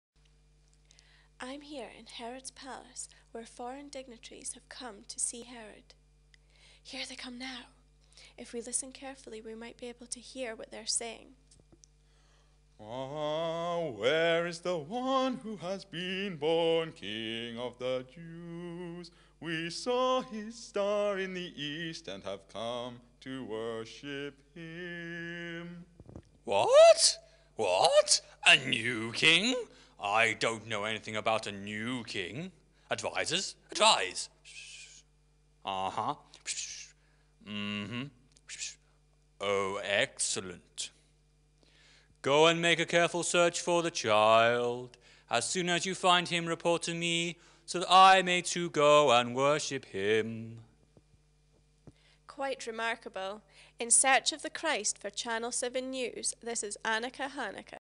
One Way Club Christmas - Palace (4 of 6)
A series of six short animations created for the One Way Club 2011 Christmas special. The children drew the images and some of the leaders provided the voices.

Written, animated and produced by Stephen P.

Voices
Annakah Hanukkah - Hannah
Estelle Wancha - Sarah
Tarry Knight - Jonathan
Ernie Awlion - Jacob
Wiseman - Jonathan
King Herod - Stephen
Nadine Winkle - Naomi

Pictures
Annakah - Mhairi
Bethlehem - Jayda
Sun - Kailyn
Hills - Hannah

Estelle - Abbie
Inn - Tamara
Donkey - Rebecca
Crowd - Caitlin, Amy & Tennaye

Tarry - Robbie
Ernie - Samuel
Sheep (on hills) - Toby & Ellen
Sheep (at front) - Jessica

Herod - Bradley
Advisors - Joshua & Maya
Wisemen - David, Jacob & Ryan

Stable - Jamie
Joseph - Melanie
Mary -
Cow - Emma
Shepherd - Daniel

Nadine - James